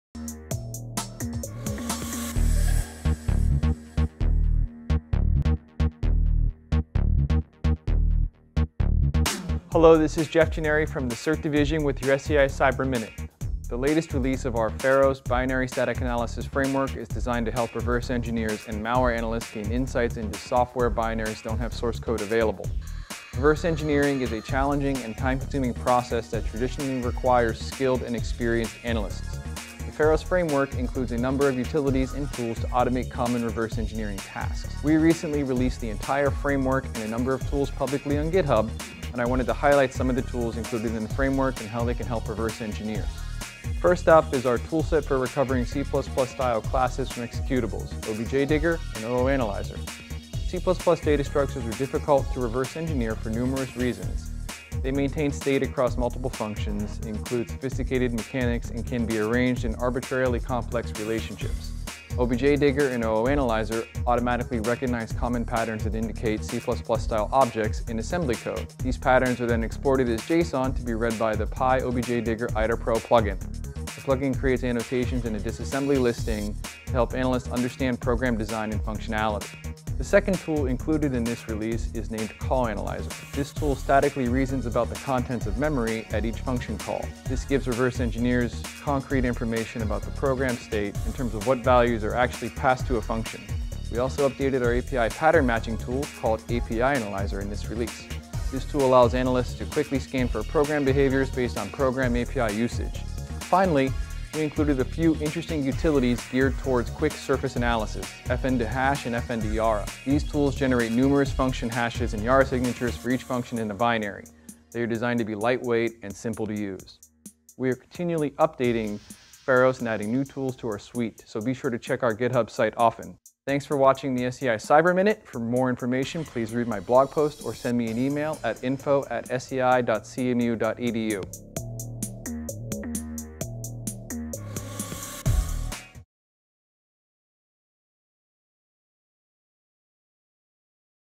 Automated Reverse Engineering with Pharos
Watch Jeff Gennari in this SEI Cyber Minute as he discusses "Automated Reverse Engineering with Pharos."